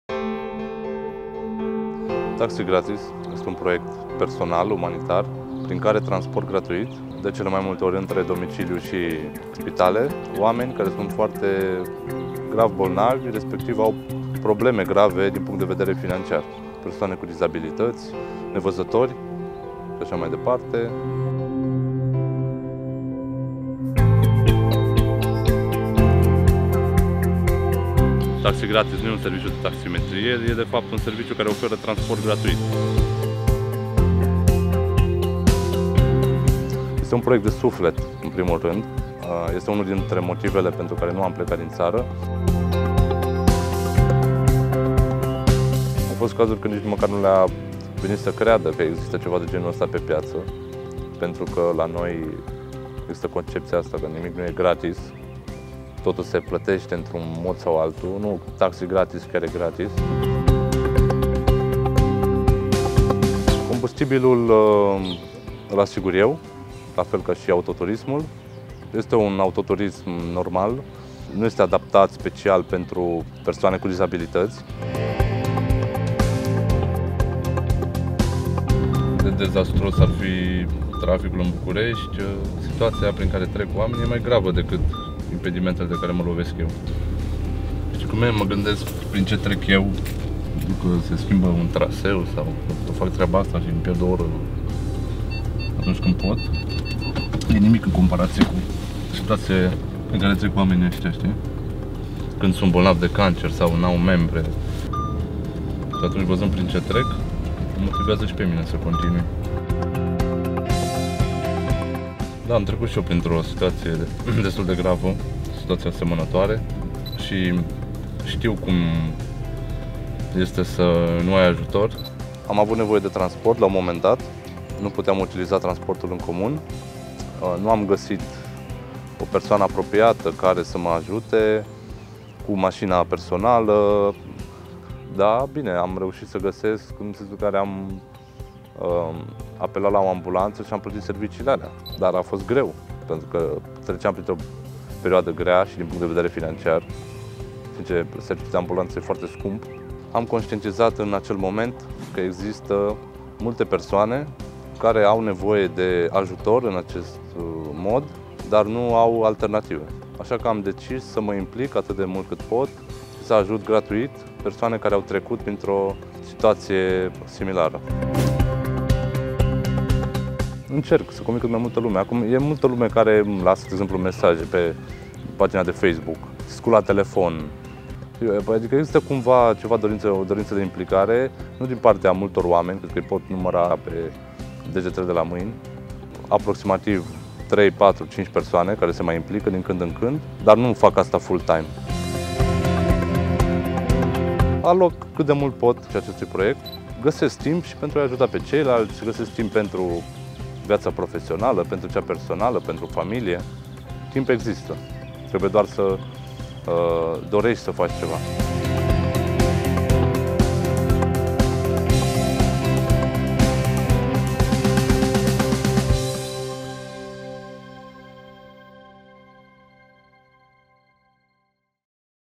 Taxi gratis: un om, o mașină și niște fapte bune
Alex Bobeș este un tânăr IT-ist care, în timpul liber, transportă gratuit, cu mașina sa, oameni bolnavi și prea săraci ca să-și permită să plătească o ambulanță sau un taxi obișnuit.

Materialul face parte din campania „Redescoperă binele”, realizată cu sprijinul Timișoreana.